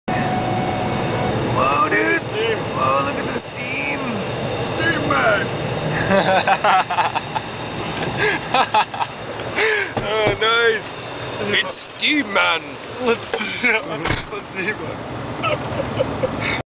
Steam Man!!!
My buddy and I were working in a very hot module in the dead of winter in Conklin, Alberta. We came outside and Josh started steaming when he took his coveralls off Haha! funny shit